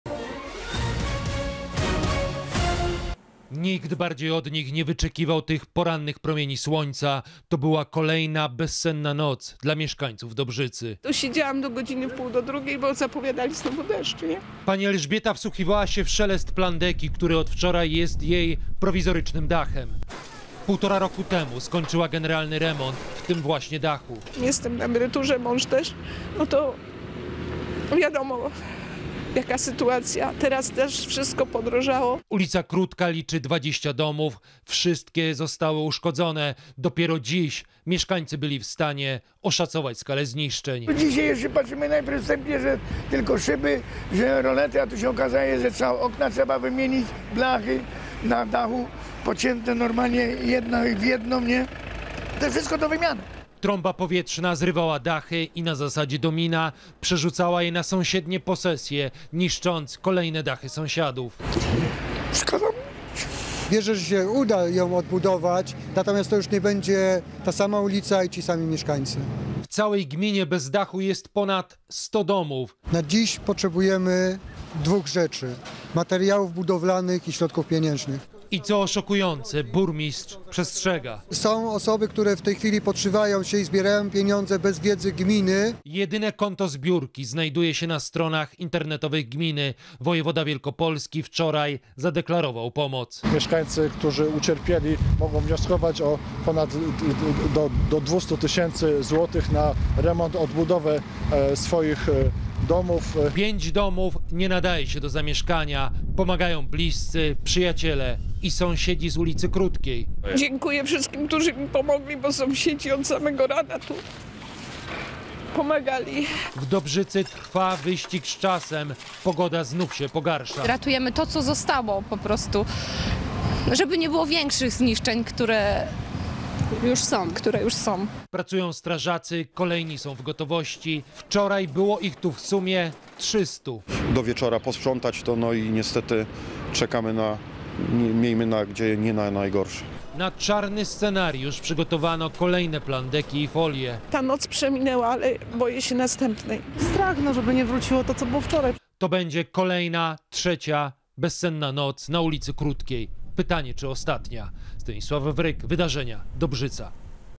Dobrzyca, doba po wichurze i chwile przed... kolejną
Wielu mieszańców wielkopolskiej Dobrzycy, najbardziej zniszczonej przez trąbę powietrzną, tej nocy nie będzie spać spokojnie. Bo wczoraj poranek przyniósł im prawdziwe spustoszenie. W liczącym zaledwie trzy i pół tysiąca mieszkańców mieście zniszczonych jest 101 budynków mieszkalnych, 65 gospodarczych. Pięć nadaje się jedynie do rozbiórki.